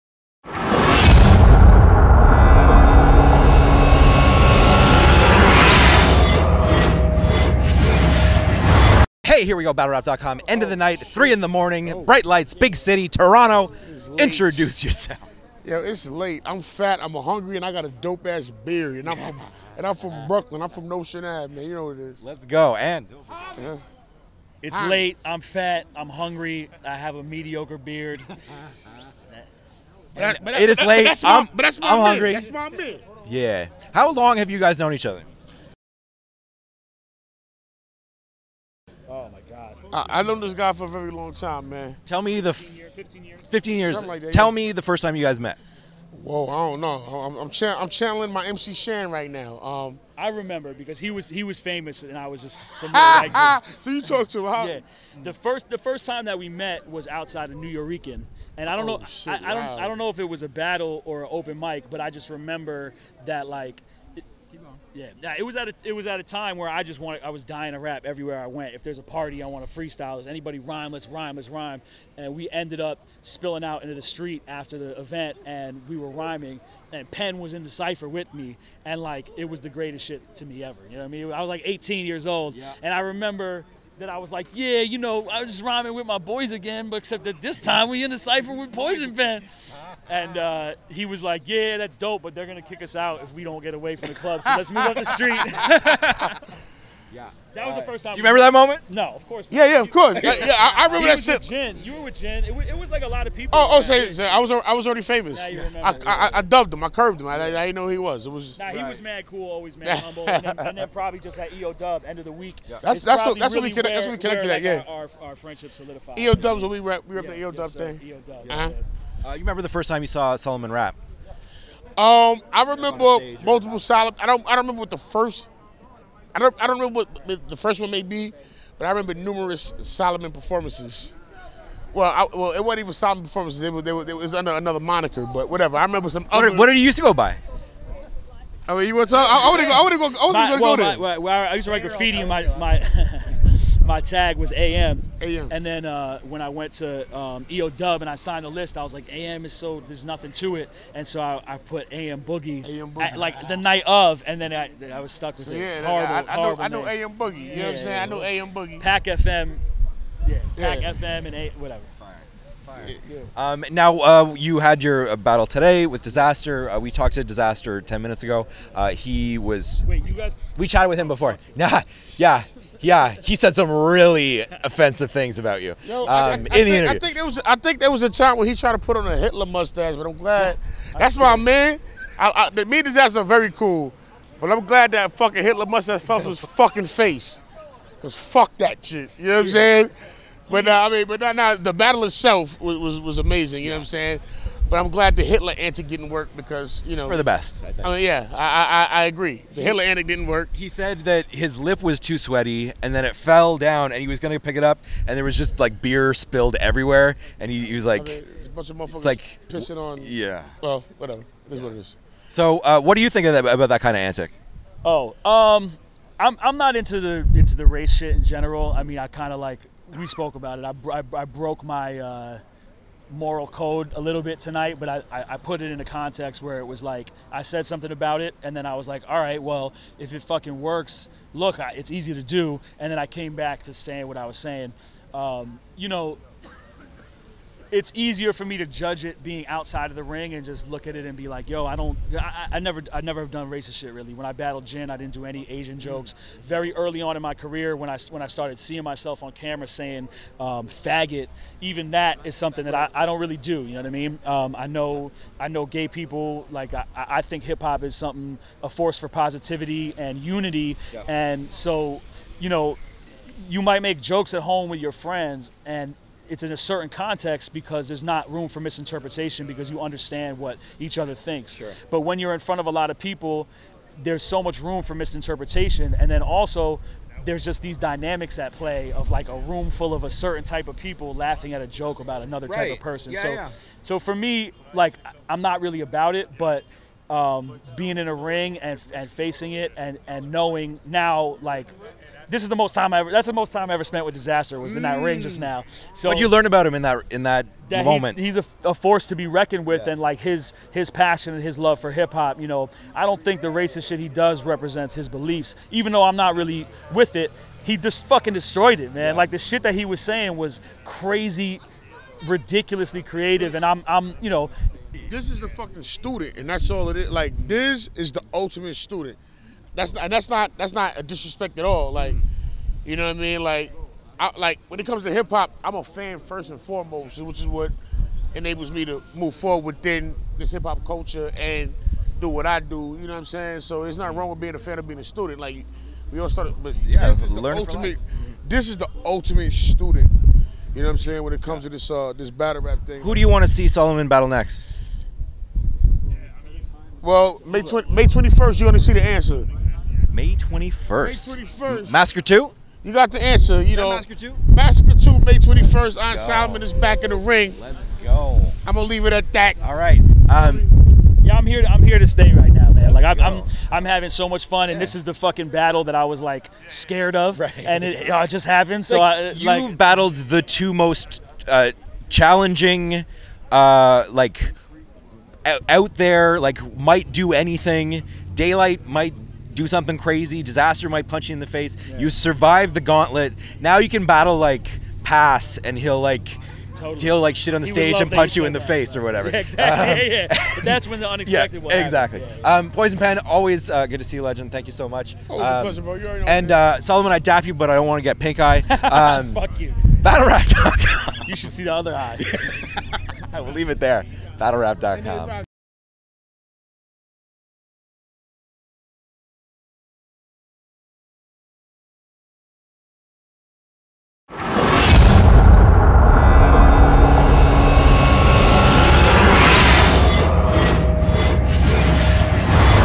Iron Solomon & Poison Pen On Battle History, Dizaster's Racial Content
Poison Pen and Iron Solomon took some time to talk with BattleRap.com after Iron's headline battle with Dizaster at KOTD's "Blackout 6" in Toronto on Feb. 19.